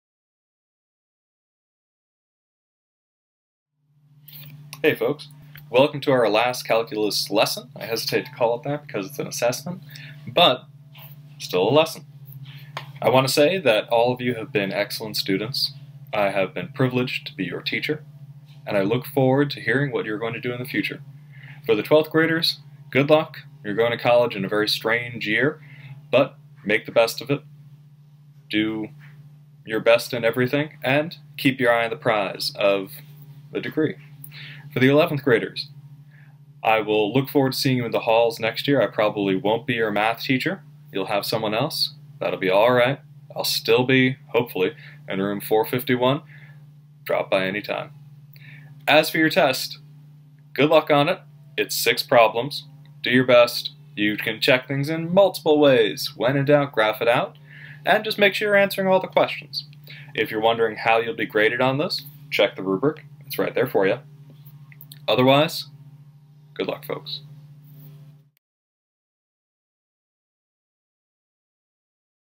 10.4b - Maximization and Minimization (part 2)
A brief introduction to a chapter exam, and a heartfelt message.